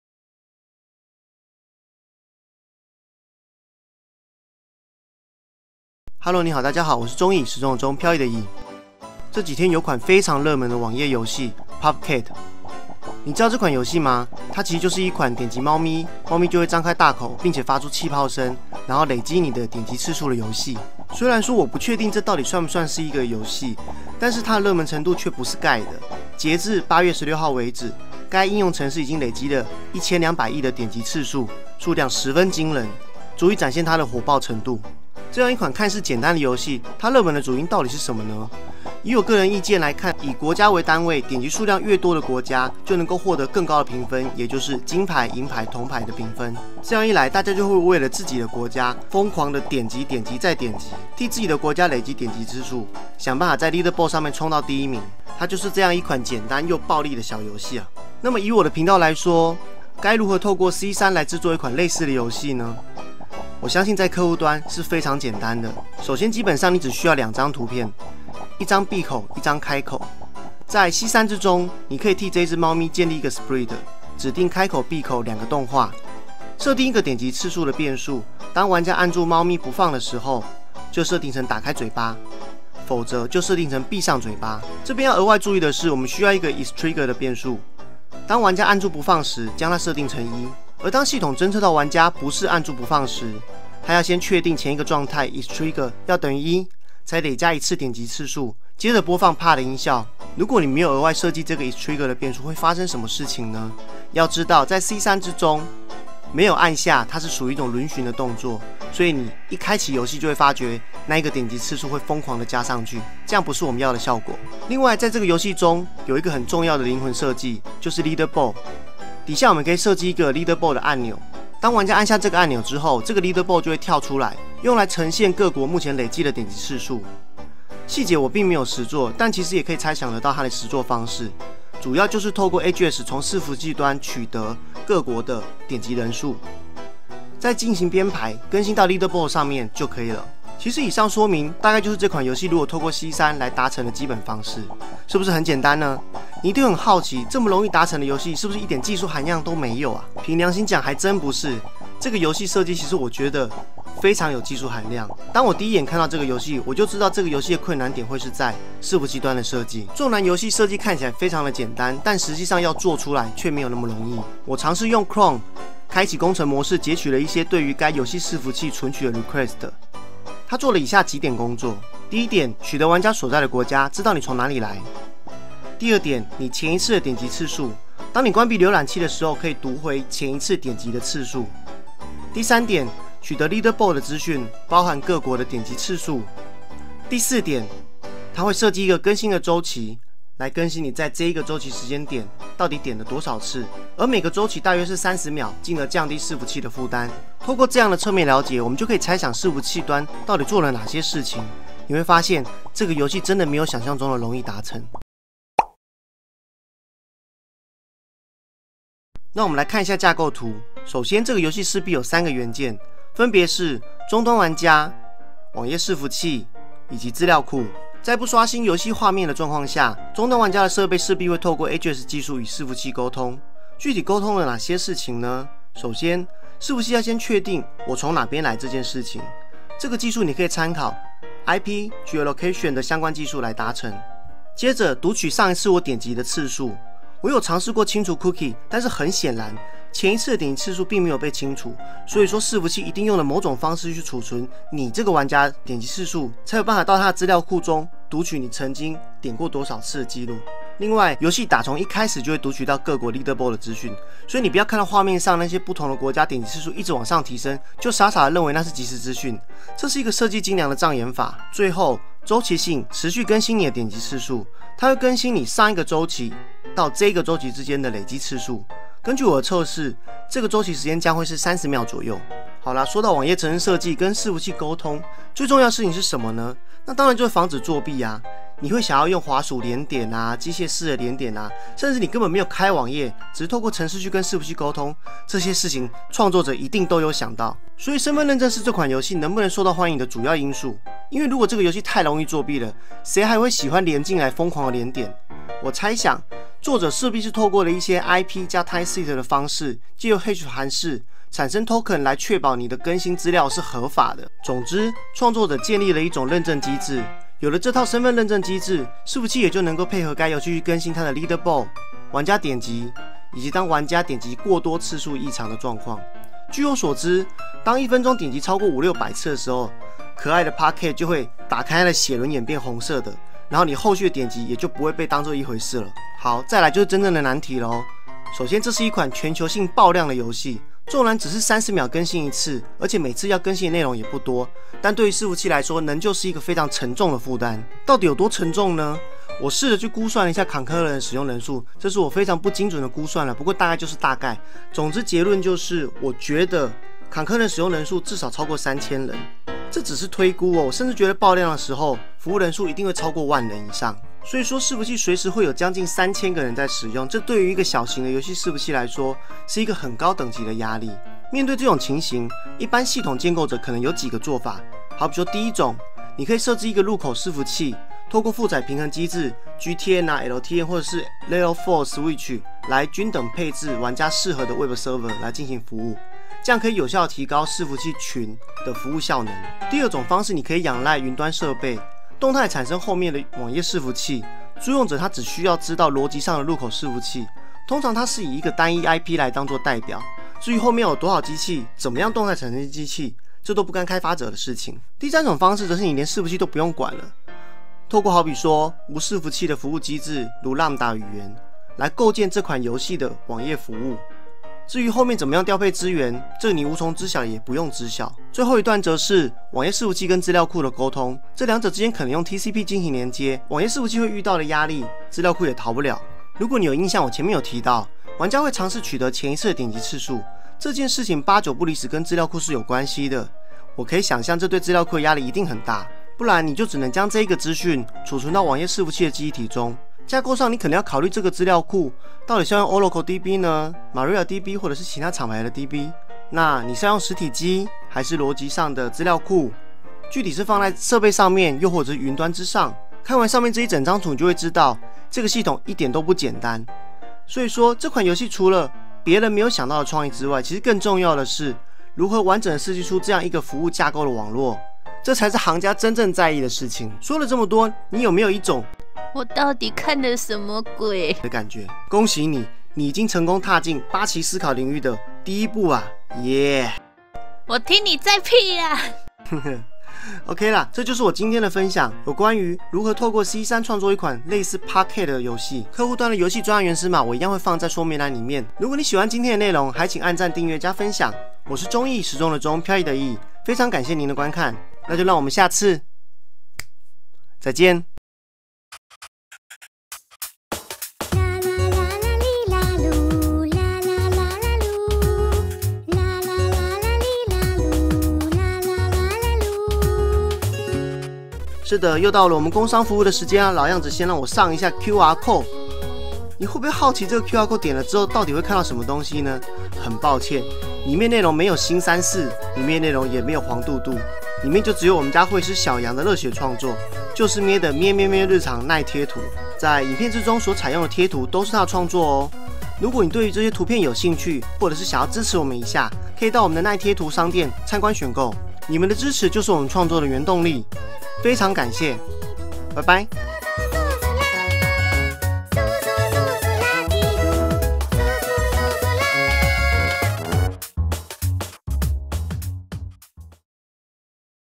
Construct 3 - 如何創作一款類似Popcat的遊戲？
這幾天有款非常熱門的網頁遊戲：Popcat
他熱門程度真不是蓋的！
你會不會好奇、這個遊戲可能是怎麼創作的？
本次影片將簡單說明我對這個遊戲的側面了解、
以及推想該怎麼透過Construct 3(客服端)做出一款這樣的遊戲！
(本次影片喉嚨狀況不太好、還請多多見諒囉！)